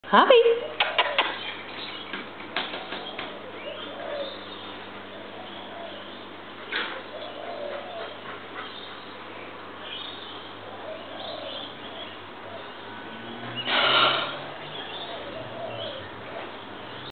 Harvey and Loki - Playing 1.
Harvey the Ragdoll kitten, and Loki the Malamute puppy playing.